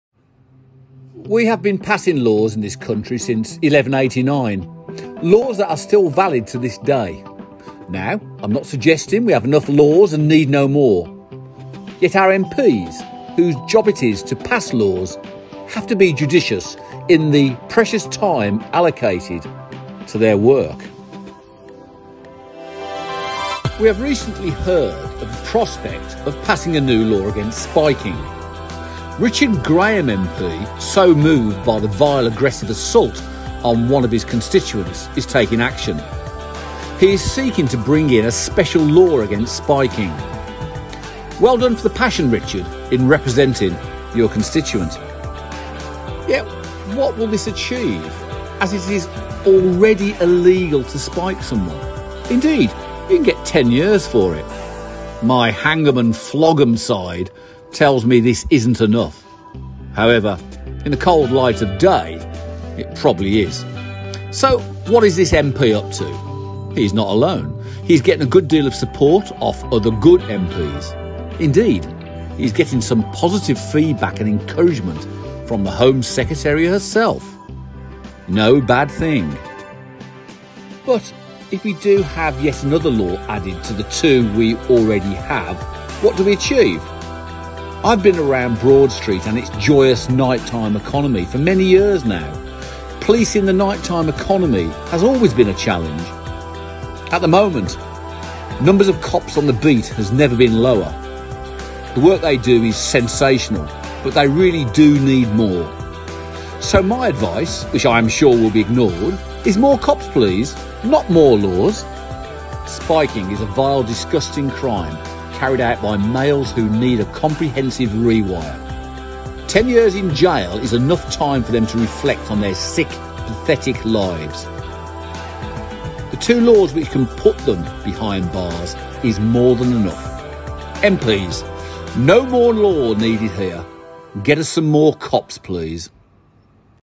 MPs: we don’t need new laws to tackle drink-spiking, but we do need more bobbies
MP Richard Graham has been so moved by how one of his constituents suffered from drink-spiking that he is campaigning for a new law. But Westside business leader MIKE OLLEY argues that what is needed is not more legislation, but extra police officers.